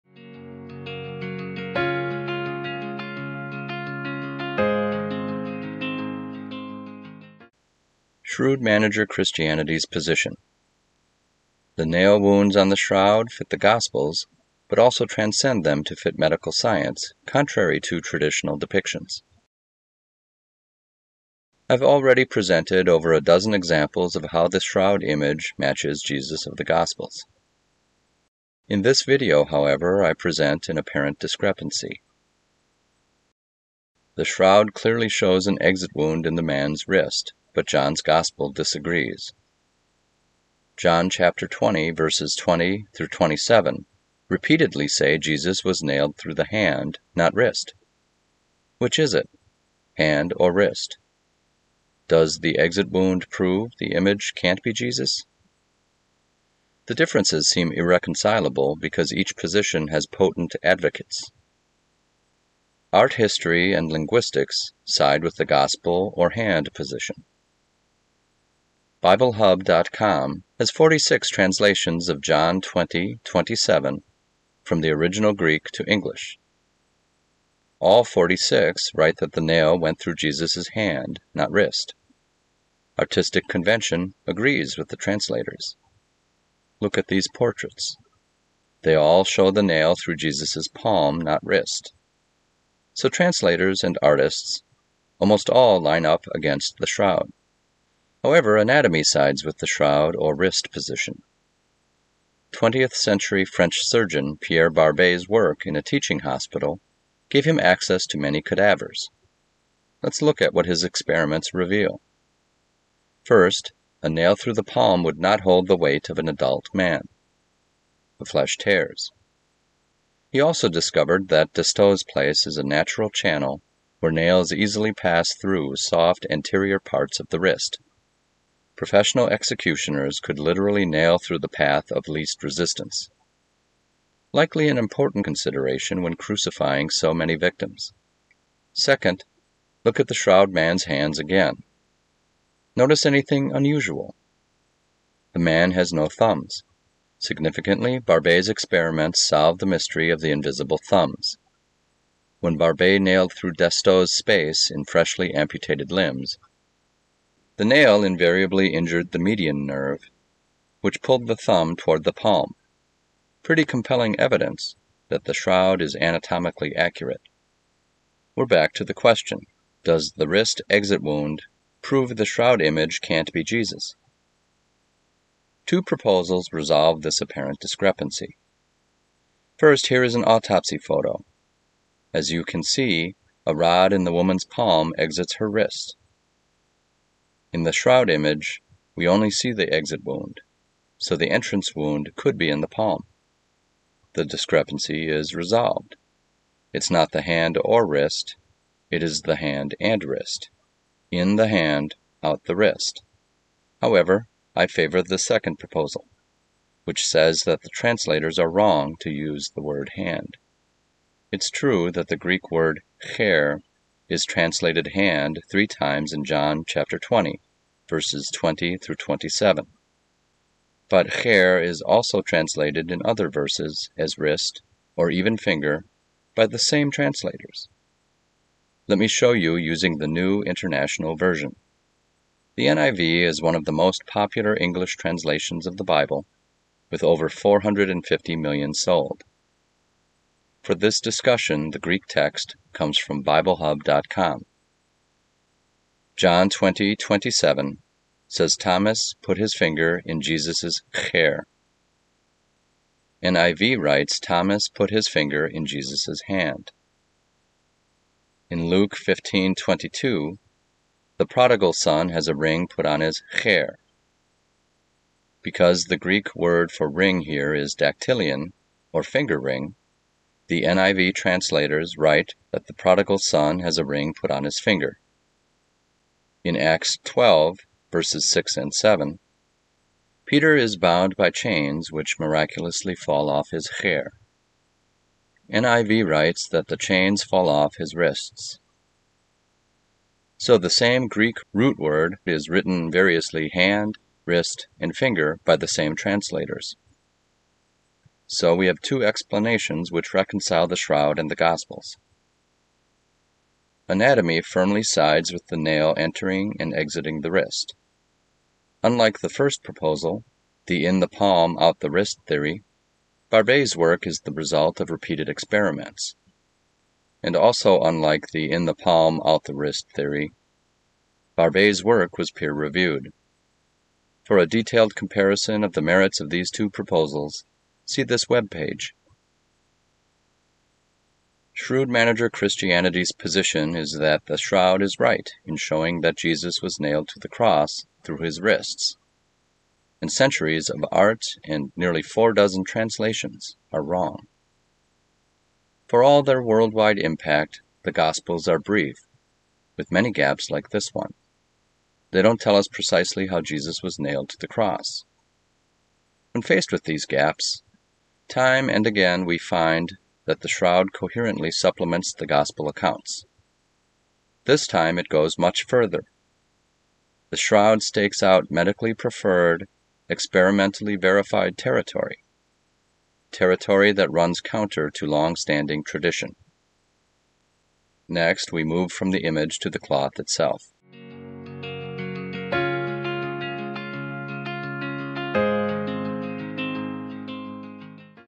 1.6 Wrist wounds on the Shroud of Turin
Medically, the Shroud of Turin is more accurate than most Christian artwork and most English Gospel translations. This is best seen in the wrist wounds and experiments on cadavers.